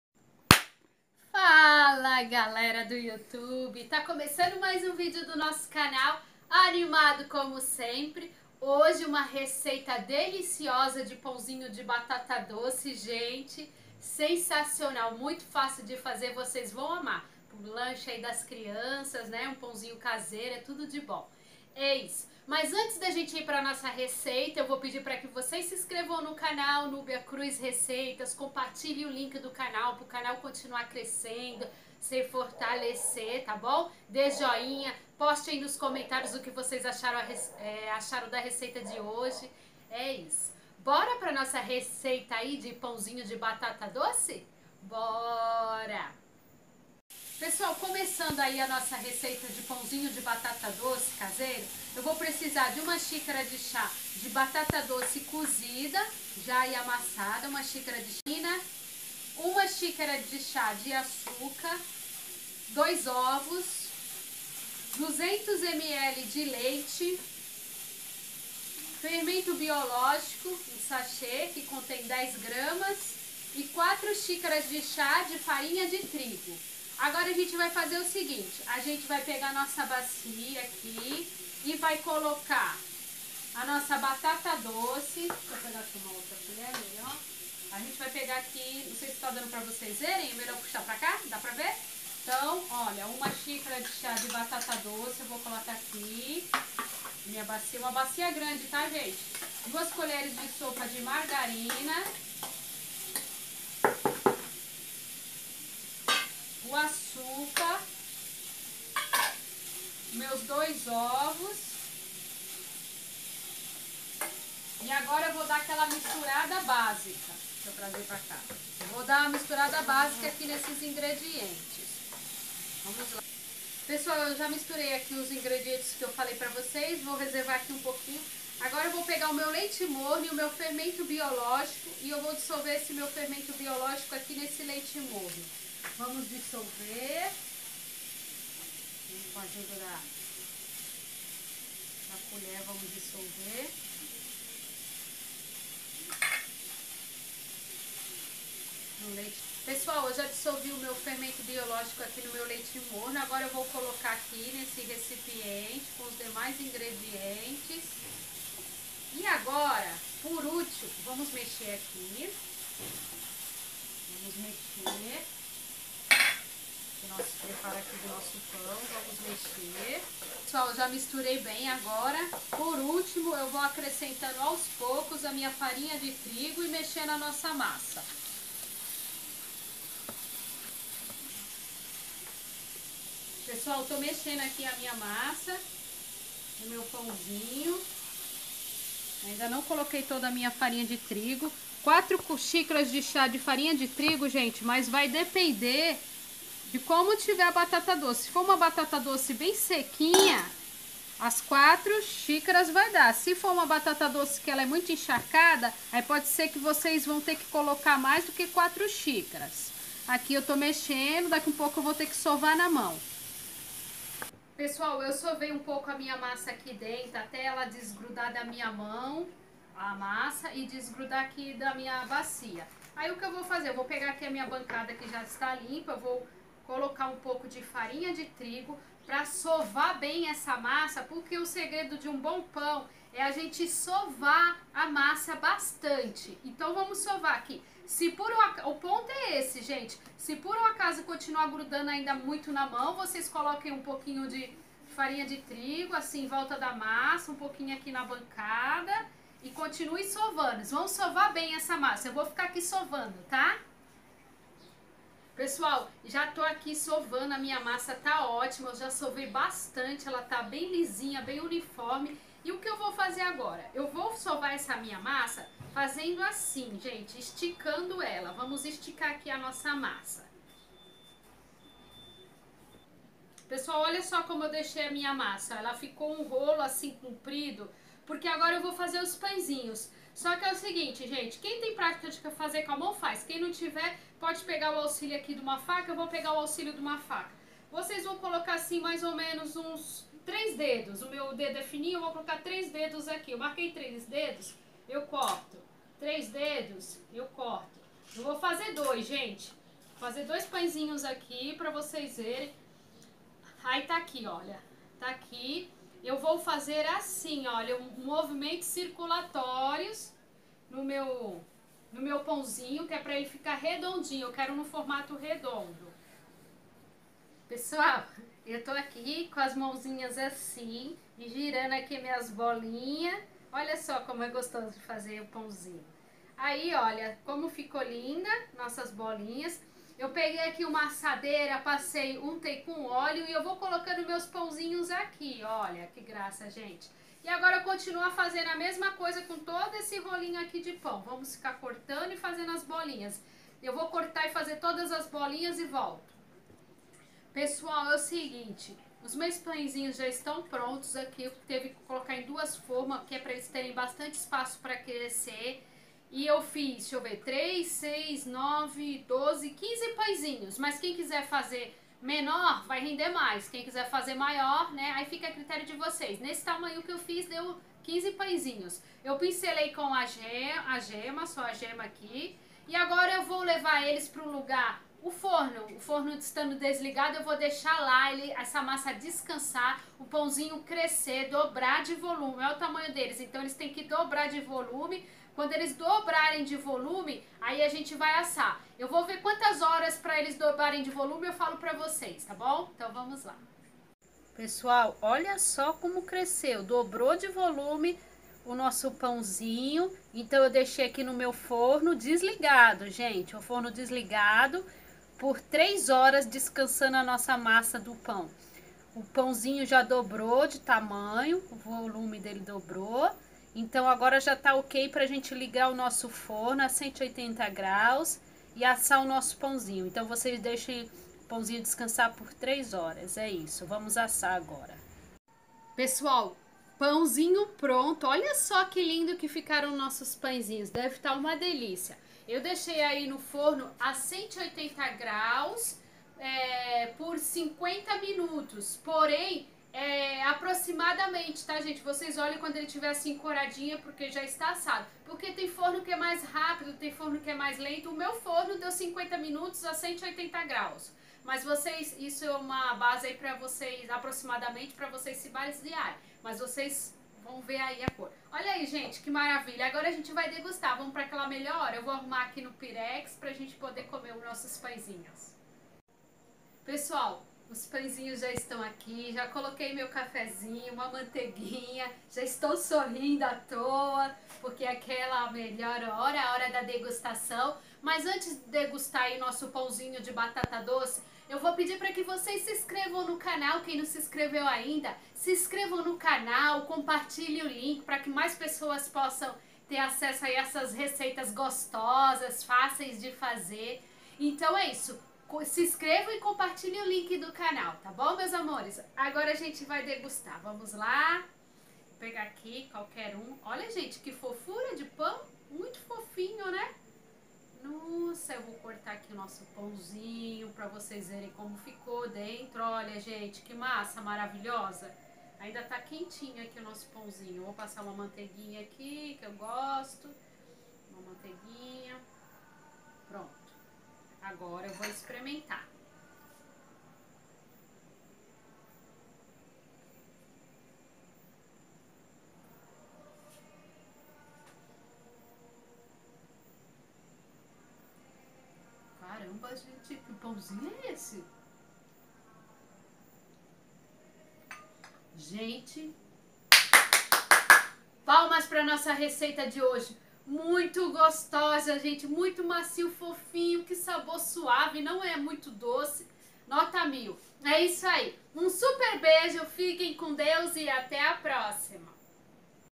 PÃO CASEIRO DE BATATA DOCE O MAIS GOSTOSO QUE JÁ COMI
CulináriaemCasa Receitasfáceis NúbiaCruzReceitas Olá pessoal , hoje uma receita deliciosa , fácil e prática de pão caseiro ,tenho certeza que vocês vão adorar .

RECEITA

1 xícara de ( chá ) de batata doce cozida e amassada
2 colheres de (sopa ) de margarina
1 xícara de ( chá ) de açúcar
2 ovos
200 ml de leite morno
10 g de fermento biológico
4 xícaras de ( chá ) de farinha de trigo

Observação,  pincele com gema em cima dos pães e quando os pãezinhos estiverem assados pincele tb com uma calda de água e açúcar.